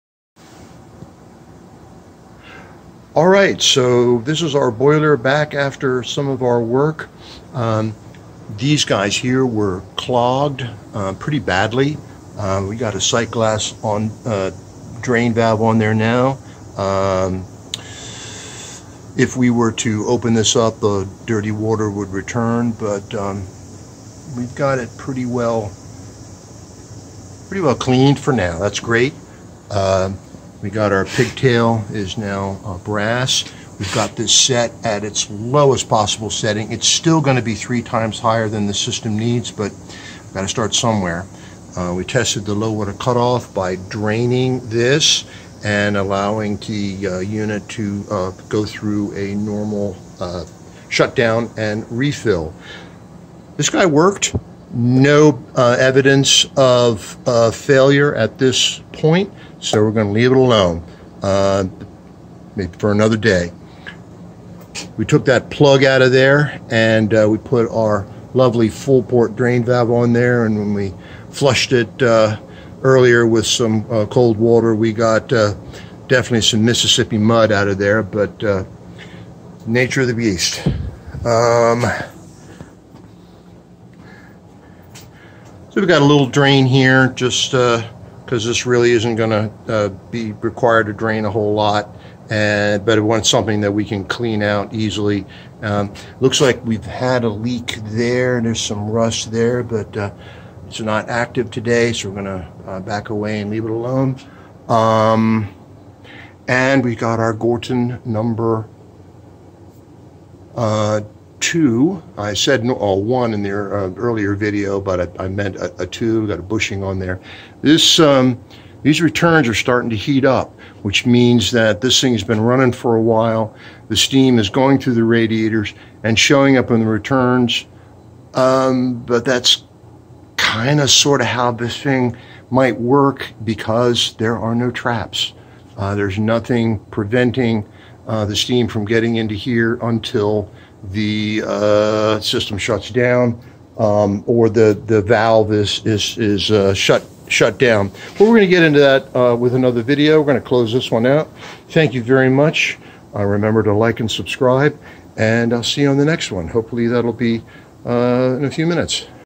Promising Results After Servicing A Steam Boiler On A Two Pipe System
This old Utica PEG150 steam boiler has seen better days, but it should work better now